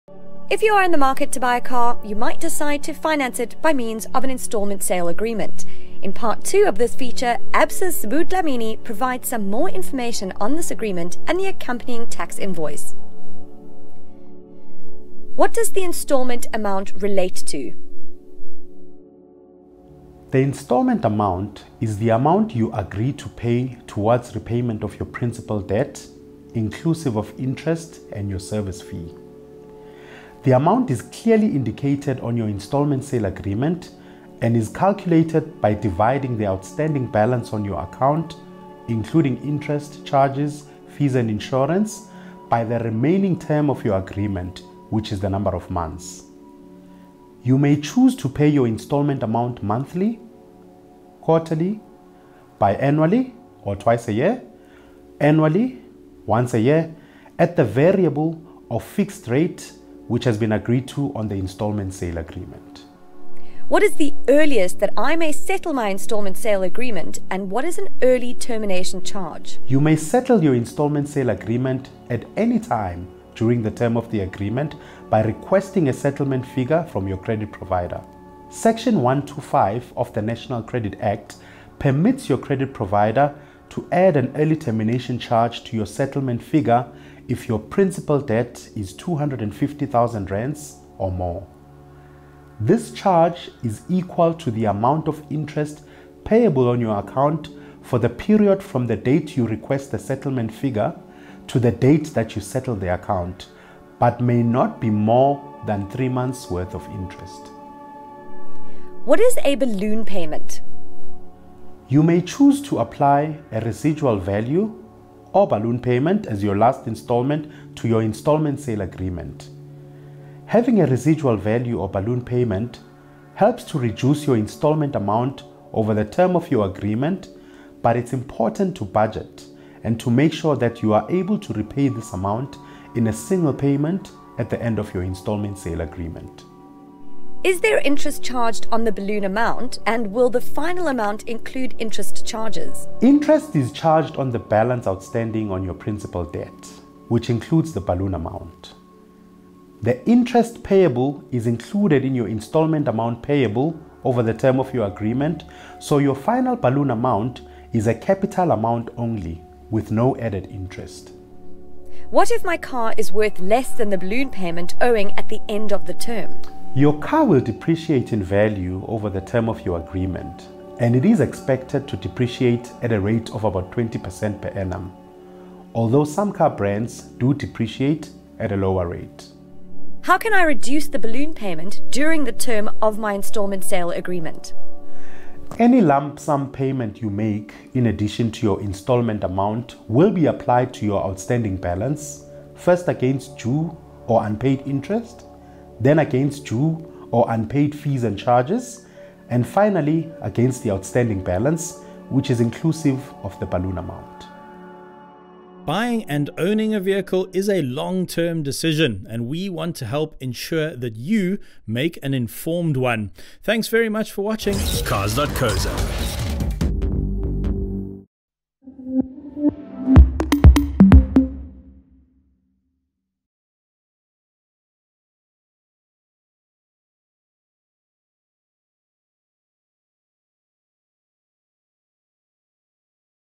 Understanding your instalment sale agreement and tax invoice, part 2
Have you decided to finance a car by means of an instalment sale agreement? Do you have an understanding of the terminology used in this agreement and the accompanying tax invoice?

In the fifth instalment of our Street Smart series and part two of this topic, Sbu Dhlamini, Head of Compliance at Absa explains some of the terms used in this agreement and the accompanying tax invoice.